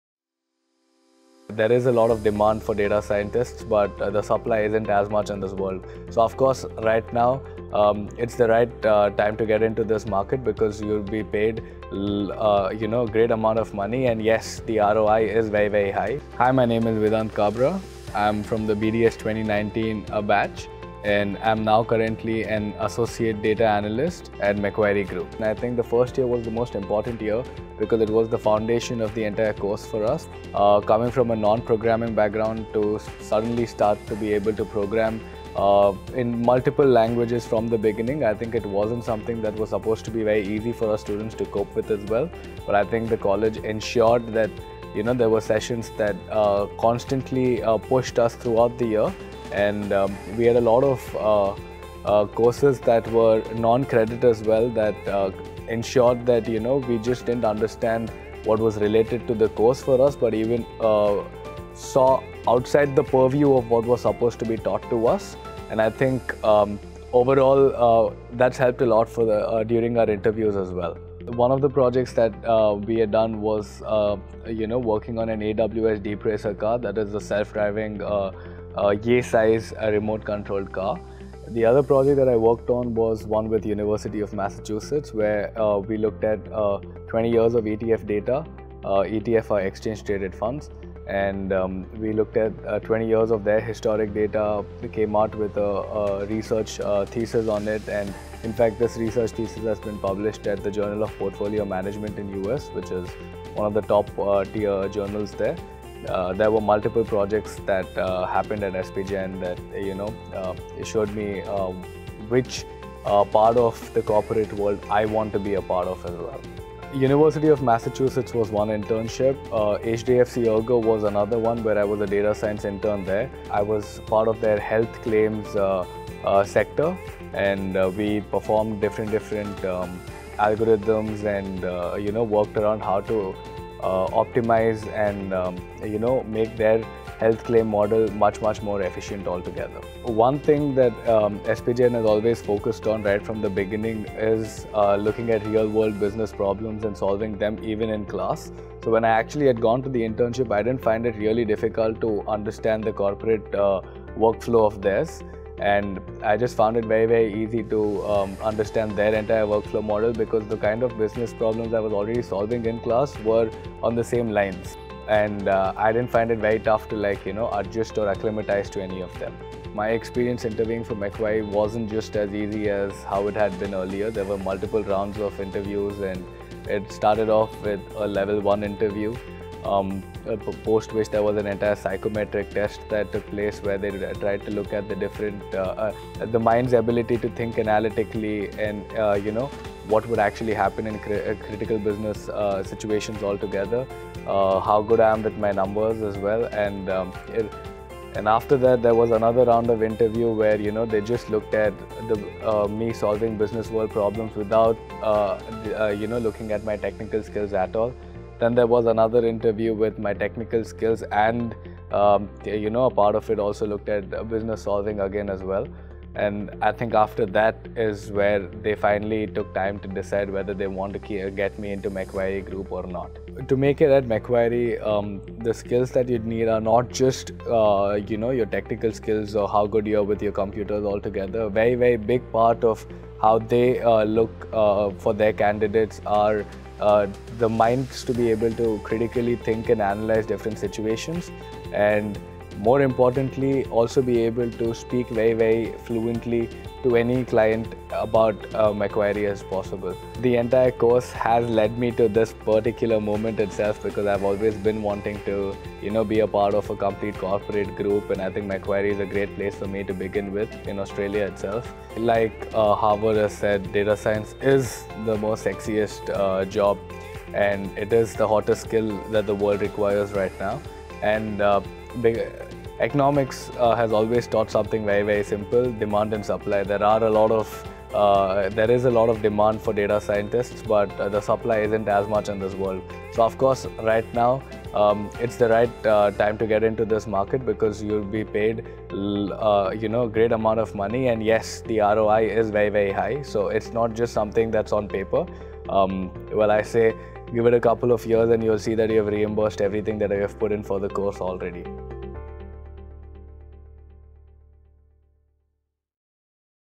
BDS Student Journey & Placement Experience: Insights from Vedant Kabra
After being a Research Intern at the University of Massachusetts Boston and a Data Science Intern at HDFC Ergo, Vedant Kabra (BDS 2022) recently landed a highly coveted full-time role with Macquarie Group in Sydney, Australia.

How did SP Jain's 3-year Bachelor of Data Science help Vedant pursue this journey?